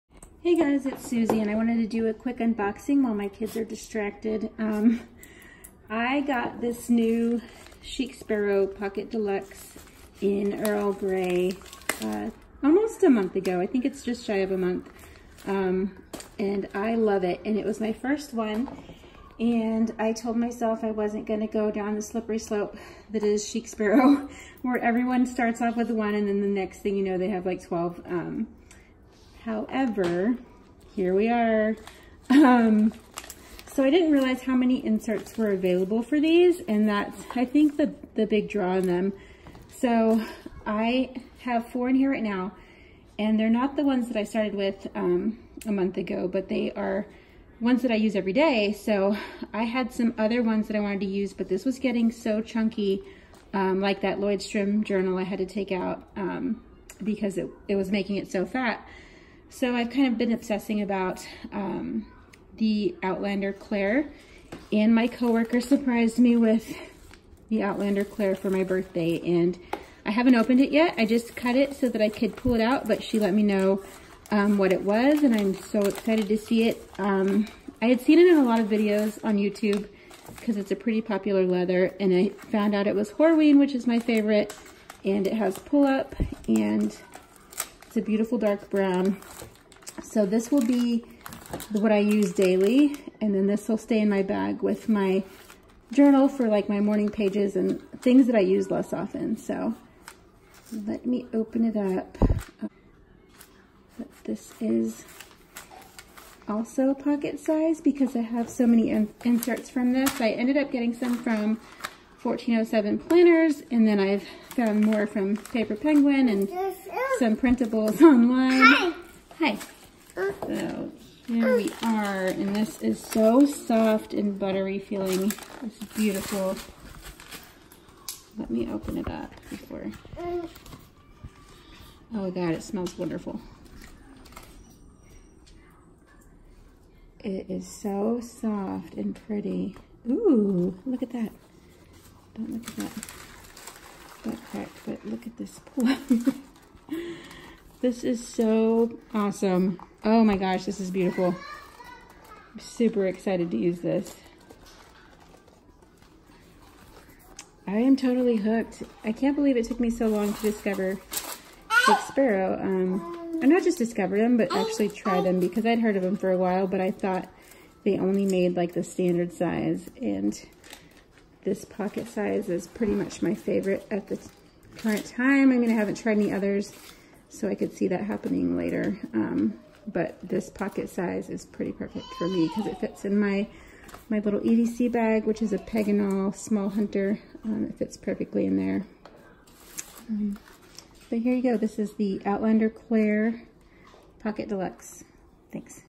Unboxing a Chic Sparrow Pocket Deluxe Outlander Claire
Super thankful to my very thoughtful friend and coworker Amanda for surprising me with a new Chic Sparrow Pocket Deluxe in Outlander Claire 😍😍😍